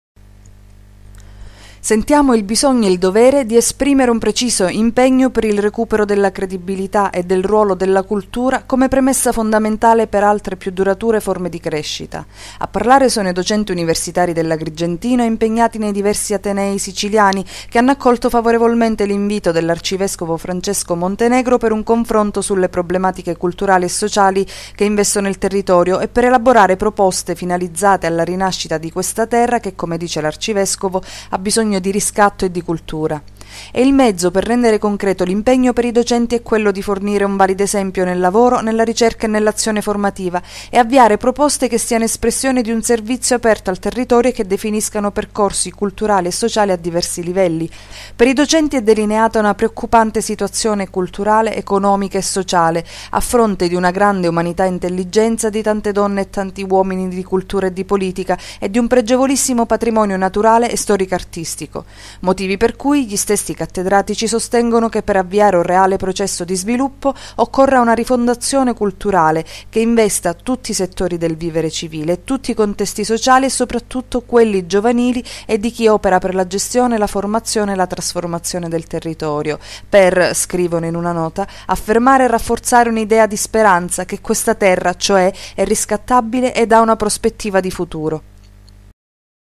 I docenti universitari rispondono a Montenegro
I docenti universitari dell'Agrigentino rispondono al recente appello di monsignor Montenegro e in un documento si dicono convinti che con il giusto impegno questo territorio sia riscattabile